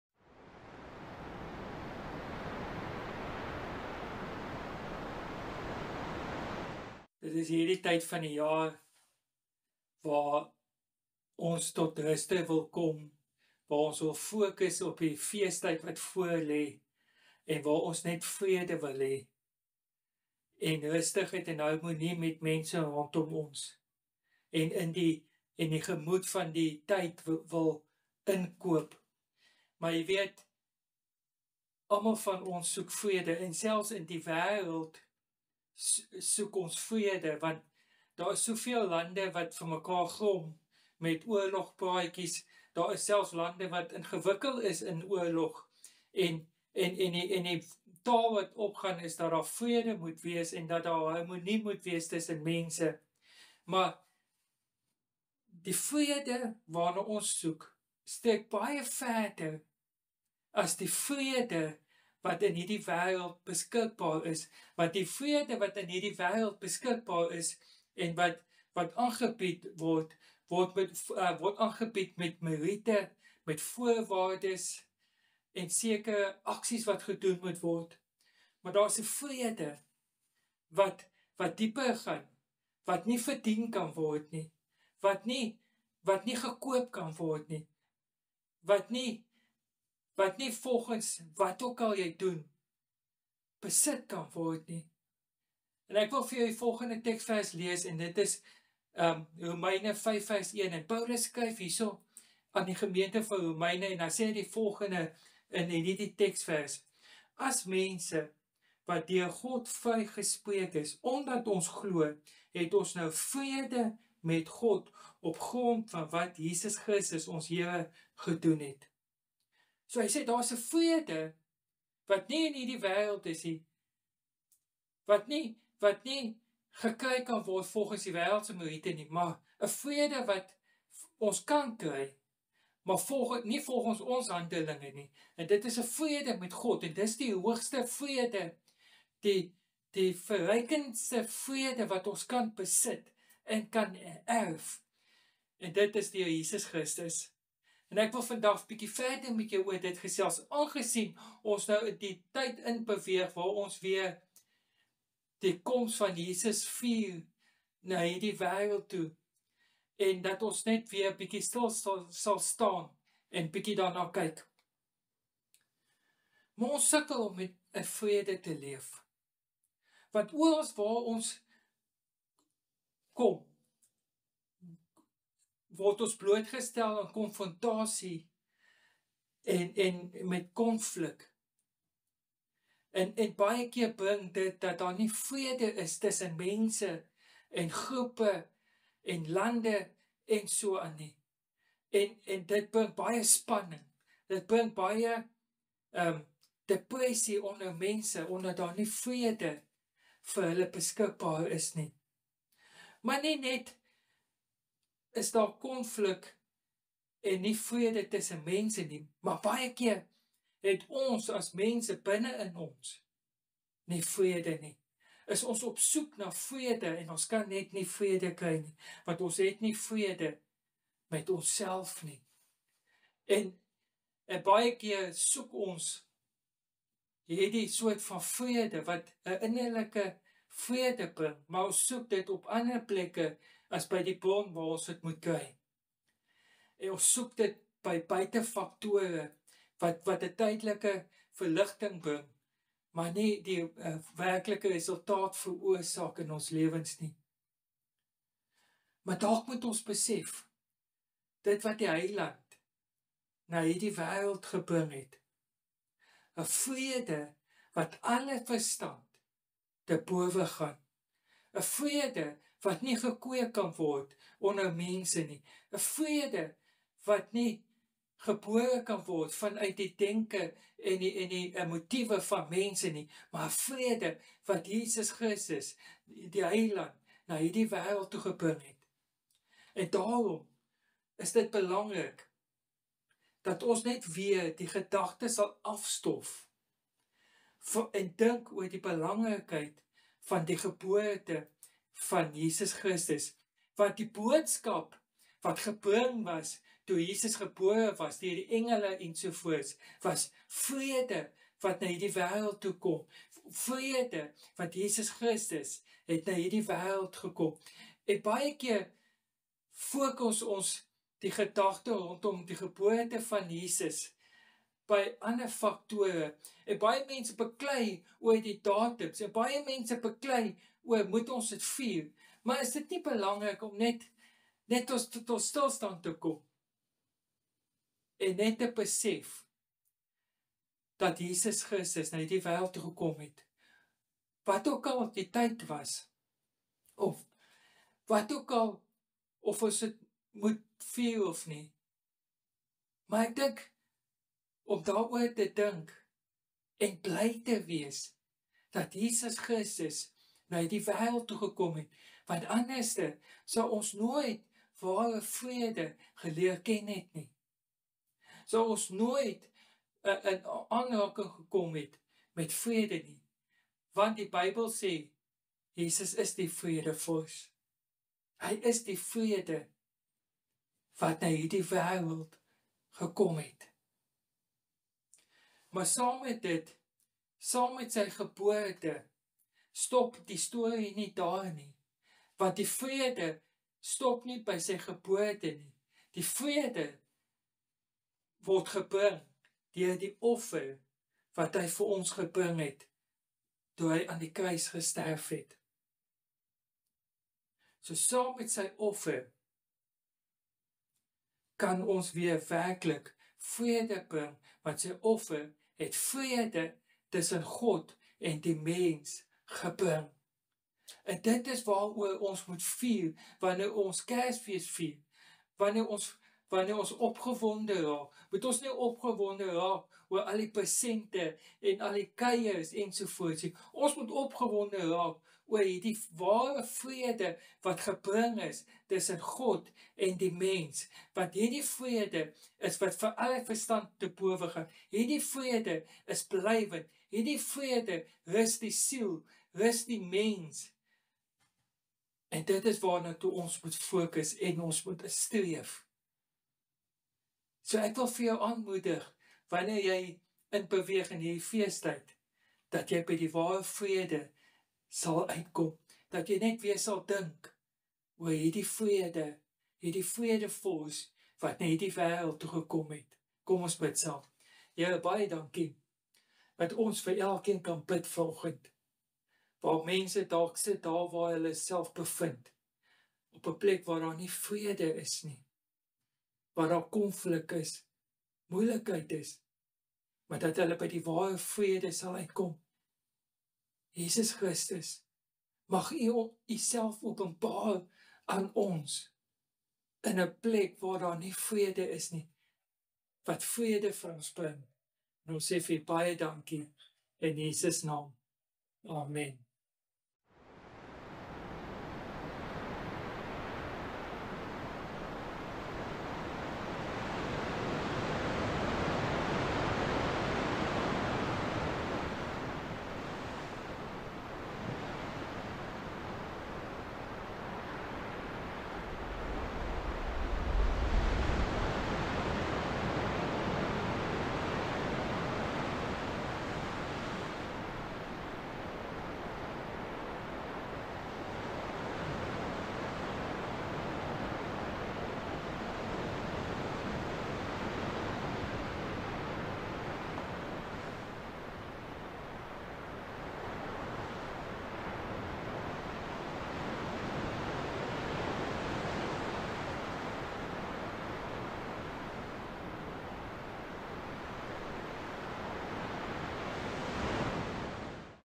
Ware Vrede
Pastoor Leon van Rensburg se boodskap van 27 November 2022. Skrifgedeelte: Romeine 5:1